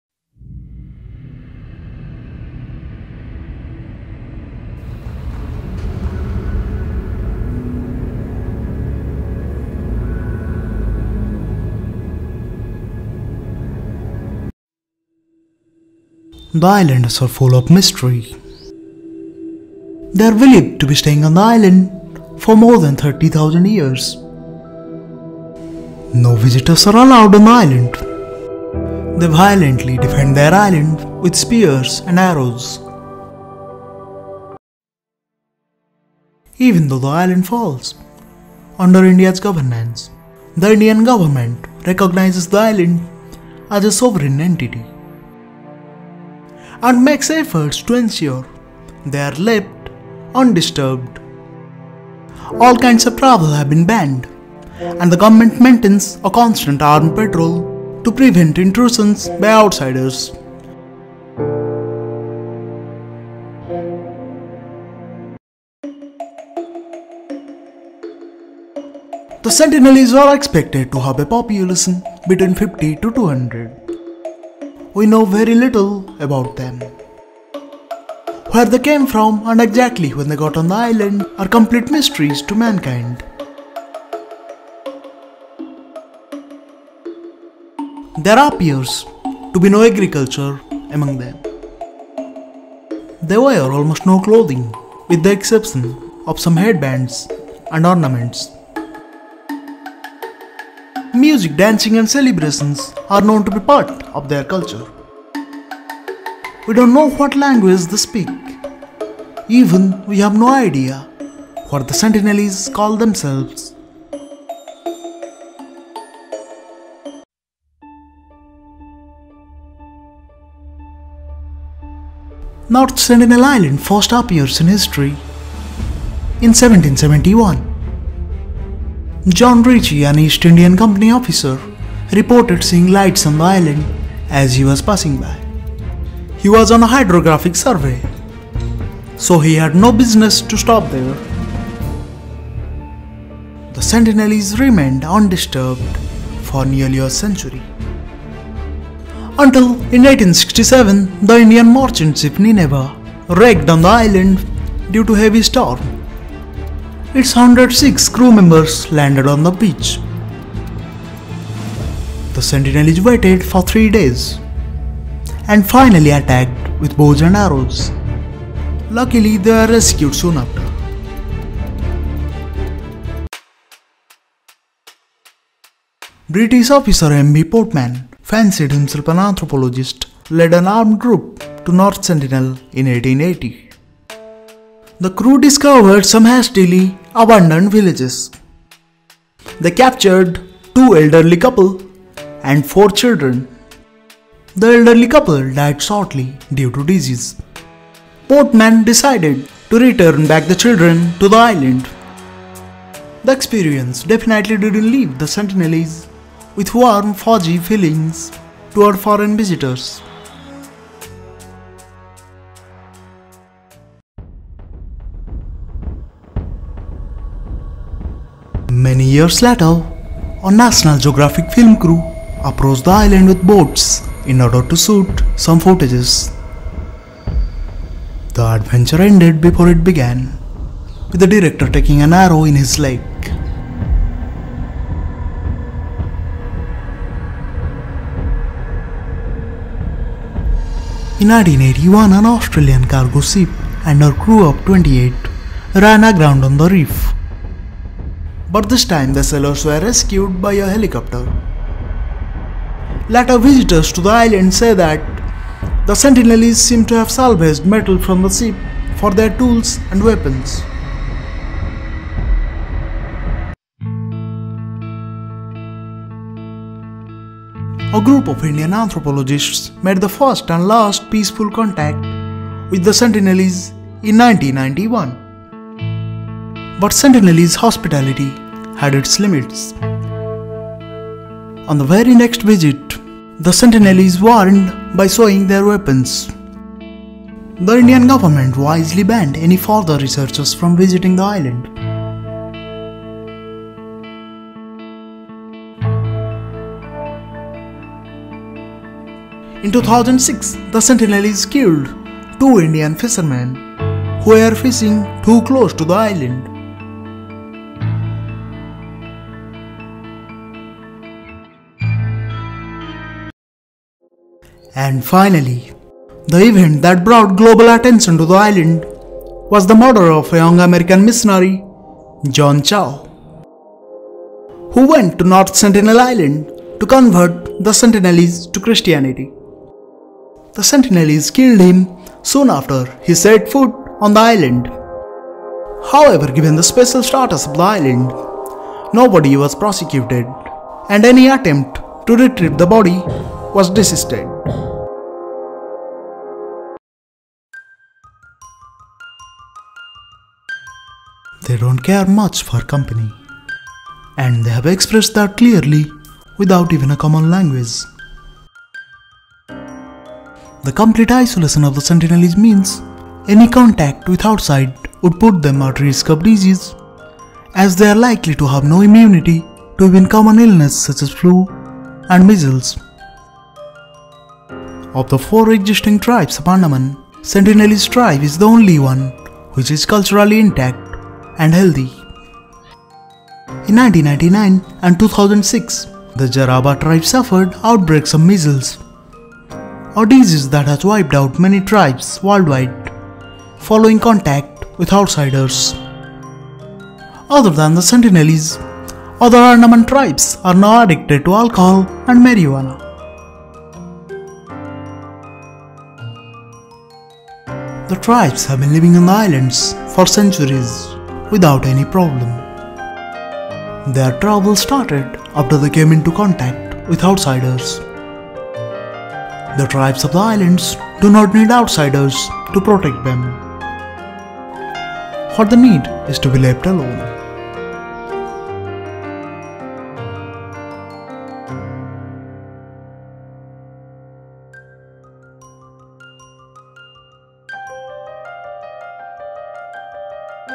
North Sentinel Island : Most Isolated Island in the World - Sentinelese Island
North Sentinel Island is located 50kms west to Andaman Islands, in the bay of Bengal. It is home to the Sentinelese people. They are widely considered the most isolated tribe in the world. The islanders are full of mystery. They are believed to be staying on the island for more than 30,000 years.

Even though the island falls under India’s governance, the Indian government recognizes the island as a sovereign entity. And makes efforts to ensure they are left undisturbed. All kinds of travel have been banned and the government maintains a constant armed patrol to prevent intrusions by outsiders.

No visitors are allowed on the island. They violently defend their island with spears and arrows. They have reputation of even shooting arrows at aircrafts that had attempted to land in their area.

The Sentinelese are expected to have population between 50 to 200. We know very little about them. Where they came from and exactly when they got on the island are complete mysteries to mankind. They live by hunting gathering and fishing. There appears to be no agriculture among them. They wear almost no clothing with the exception of some headbands and ornaments. Music, dancing and celebrations are known to be part of their culture. We don’t know what language they speak. Even we have no idea what the Sentinelese call themselves.

North Sentinel Island first appears in history in 1771. John Ritchie an East Indian company officer, reported seeing lights on the island as he was passing by. The Sentinelese remained undisturbed for nearly a century. In 1867, the Indian merchant ship Ninevah wrecked on the island due to heavy storm. Its 106 crew members landed on the beach. The sentinelese waited for 3 days and finally attacked with bows and arrows.

A national geographic film crew approached the island with boats in order to shoot some footages. The adventure ended before it began, with the director taking an arrow in his leg.
In 1981, an Australian cargo ship and her crew of 28 ran aground on the reef. But this time the sailors were rescued by helicopter. Later visitors to the island say that the sentinelese seemed to have salvaged metal from the ship for their tools and weapons.
A group of Indian anthropologists made the first and last peaceful contact with the sentinelese in 1991. On the very next visit, the sentinelese warned by showing their weapons. The Indian government wisely banned any further researchers from visiting the island.

In 2006, the sentinelese killed two Indian fishermen who were fishing too close to the island. Later the sentinelese attacked the helicopter and the crew who came to recover the bodies. Indian authorities canceled the operation.
And finally the event that brought global attention to the island was the murder of a young American missionary John Chau, who went to North Sentinel Island to convert the Sentinelese to Christianity. The sentinelese killed him soon after he set foot on the island. However, given the special status of the island, nobody was prosecuted and any attempt to retrieve the body was desisted.

They don’t care much for company and they have expressed that clearly without even a common language. The complete isolation of the sentinelese means any contact with the outside would put them at the risk of disease as they are likely to have no immunity to even common illness such as flu and measles. Of the four existing tribes of Andaman, sentinelese tribe is the only one which is culturally intact and healthy. In 1999 and 2006 the Jarawa tribe suffered outbreaks of measles, a disease that has wiped out many tribes worldwide following contact with outsiders. Other Andaman tribes are now addicted to alcohol and marijuana.
The tribes have been living on the islands for centuries without any problem. Their trouble started after they came into contact with outsiders. The tribes of the islands do not need outsiders to protect them, what they need is to be left alone.